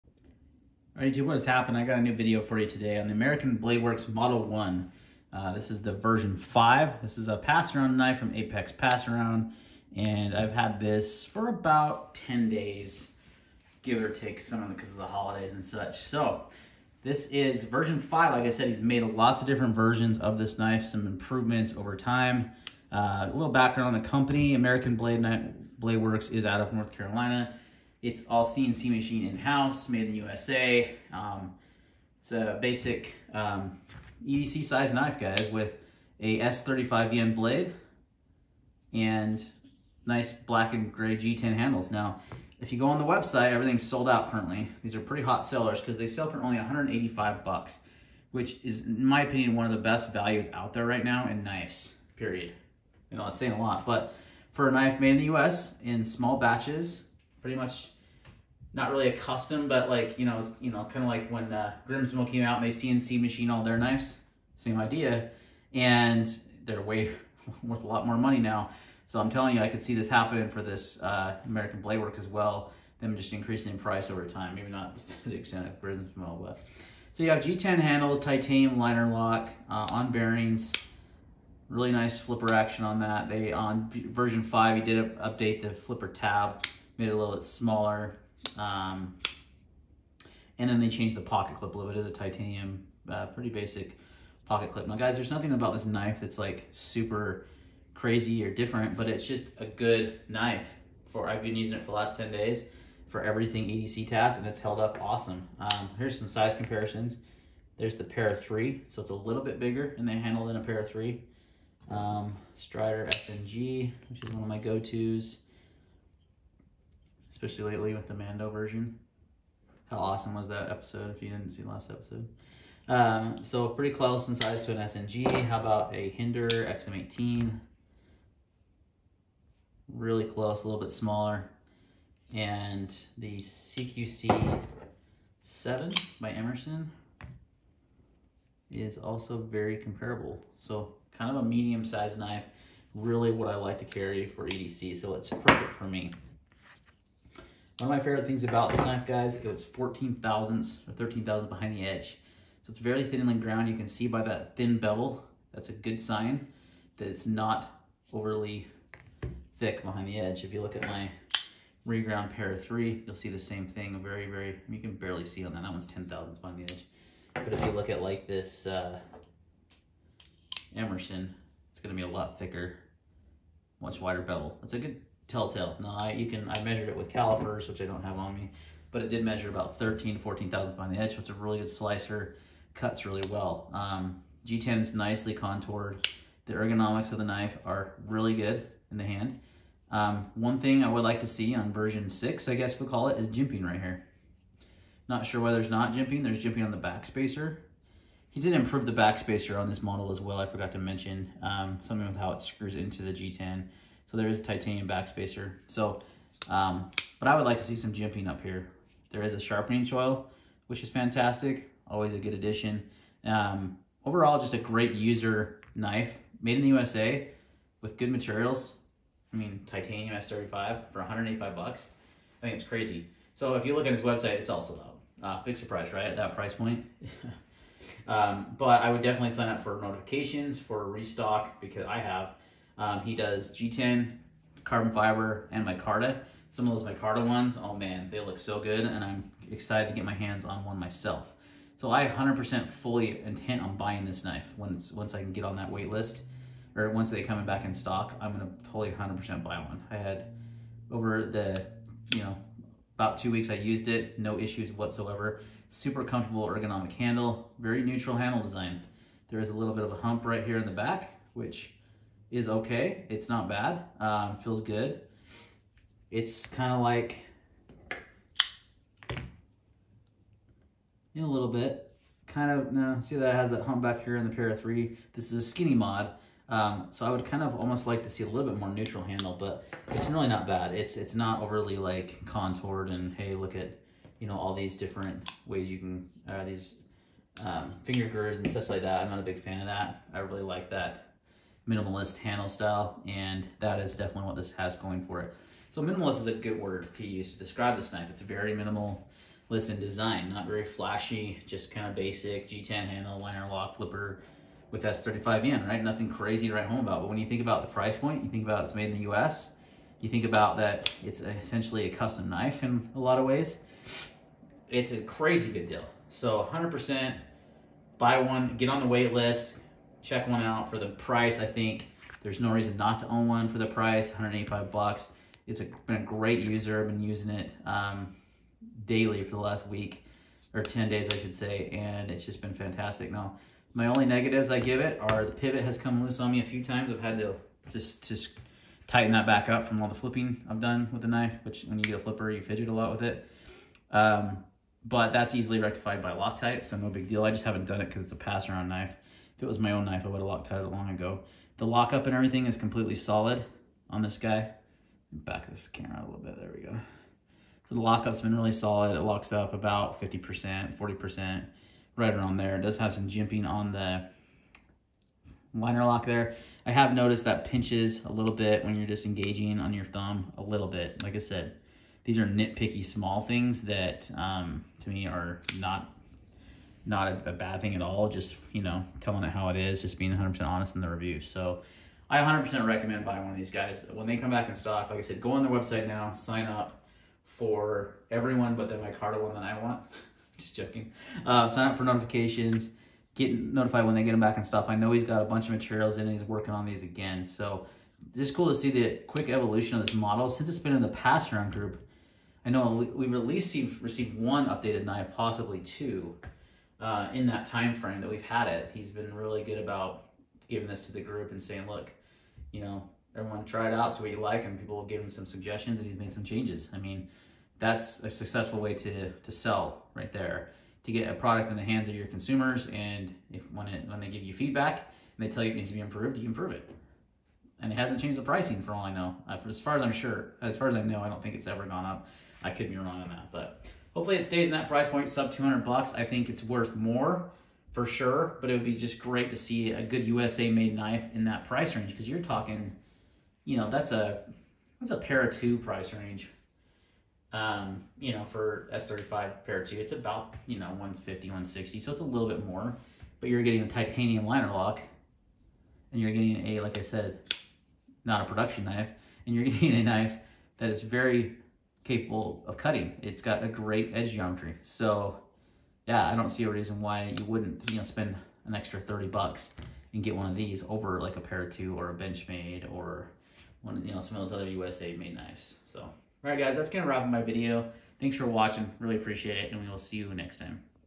American Blade Works Model 1 V5-Apex Pass Around Review
In this video I revise the American Bladeworks model 1